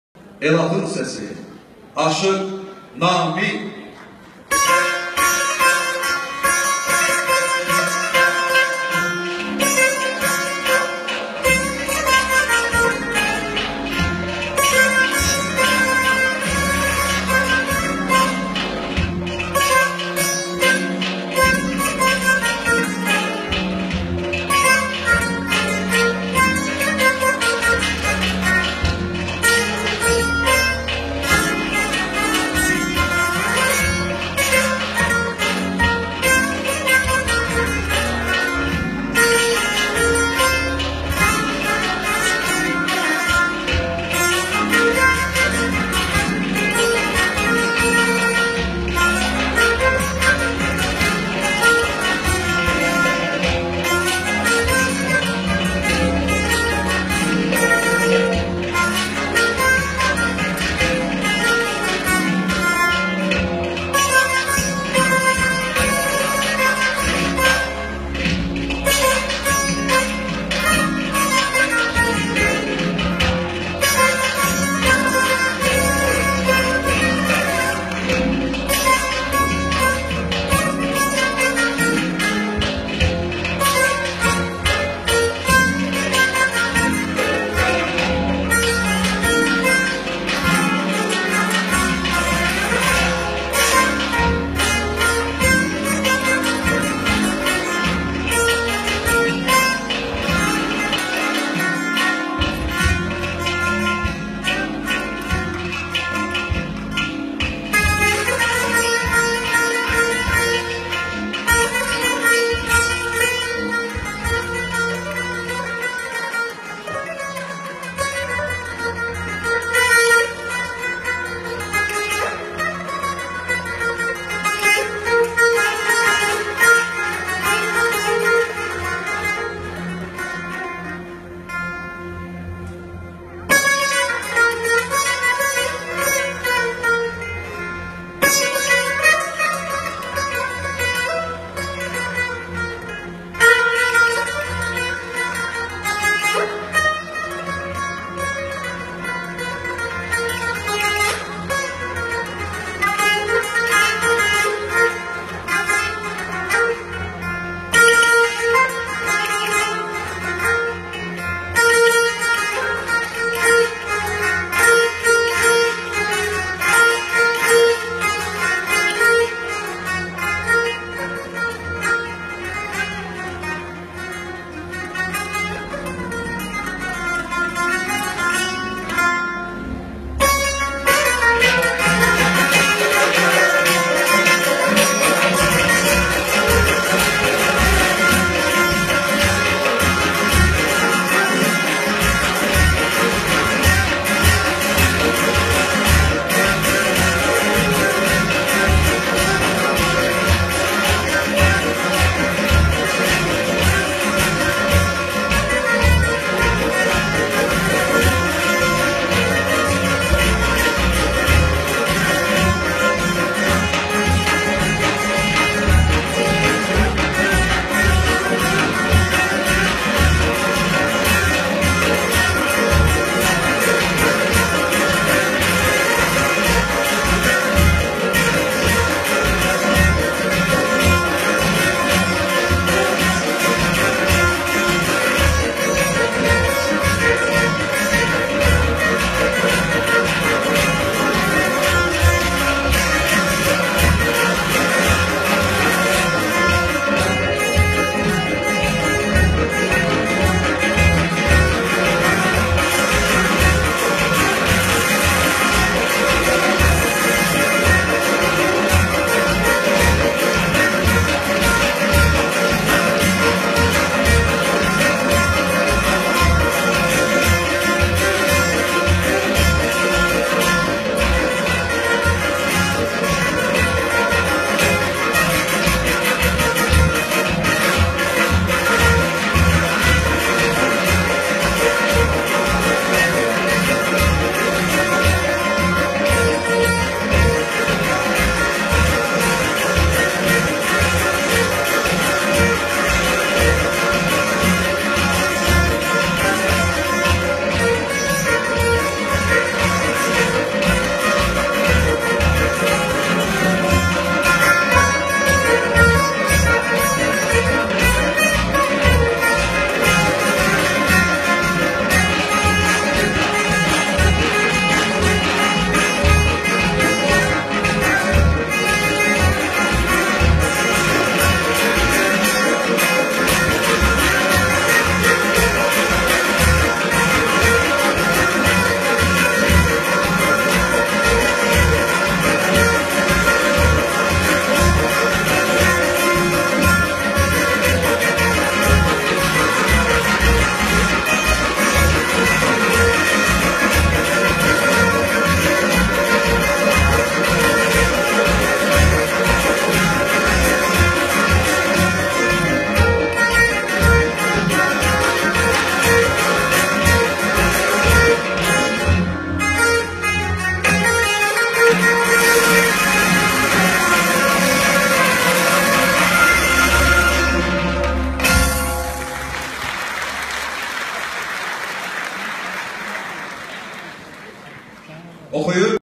Aşıq Namiq Fərhadoğlu Heydər Əliyev Sarayındakı konsertdə Yeni 2017
Hörmətli izləyicilər aşıq Namiq Fərhadoğlunun rəsmi kanalına xoş gəlmisiniz. DİQQƏT! Müəllif Hüquqları Qorunur! Bütün Hüquqlar "TiVi Media"-ya Məxsusdur. Təkrar Yayım Qadağan Edilir!